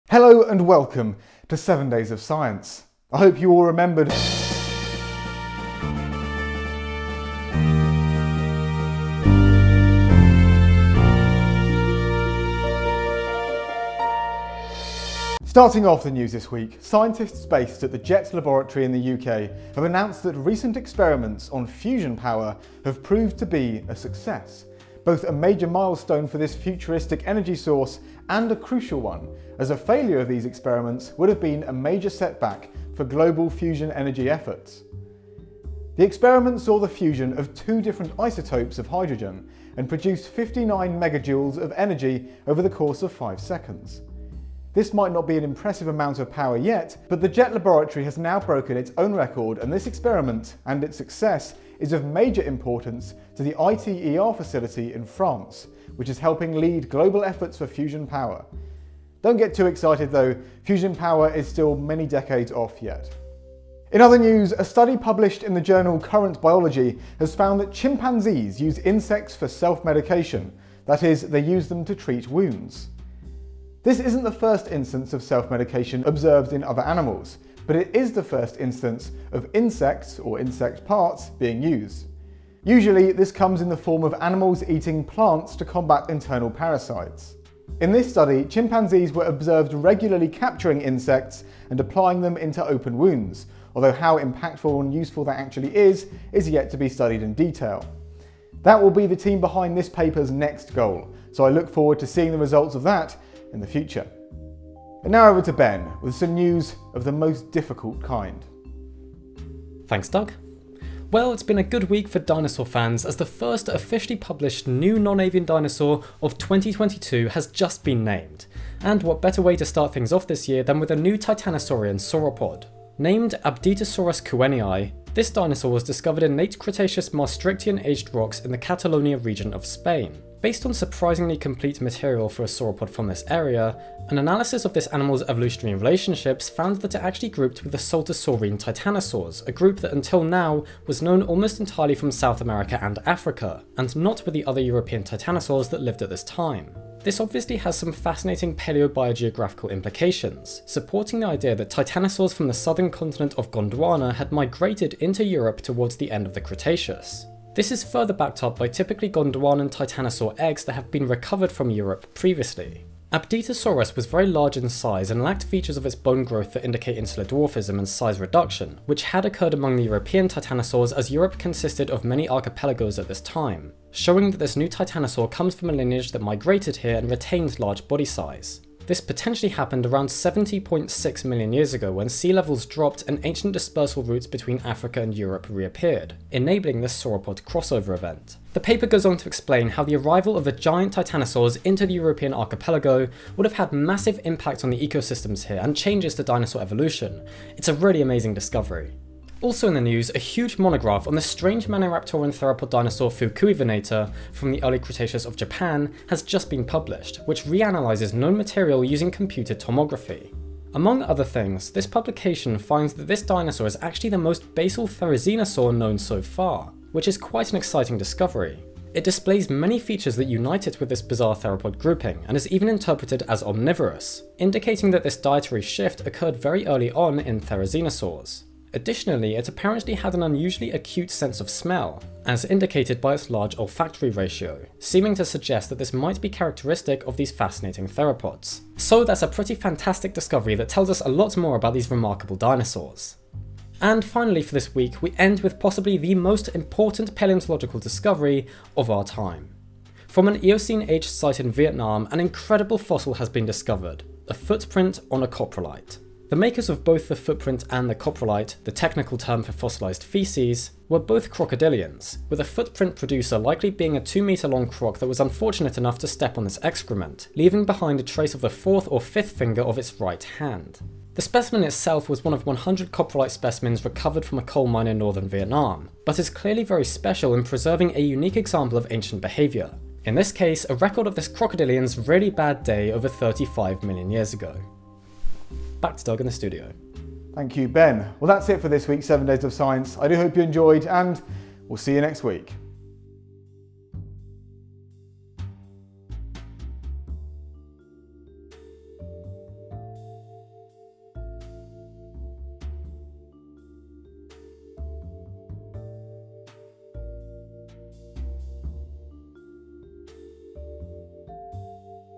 The First Therizinosaur Revealed | 7 Days of Science
In this week's 7 Days of Science, we delve into the last seven days of science. This includes fusion energy news. Also news about chimpanzees. Also something about a titanosaur. I believe a Therizinosaur also makes an appearance.
Also poo.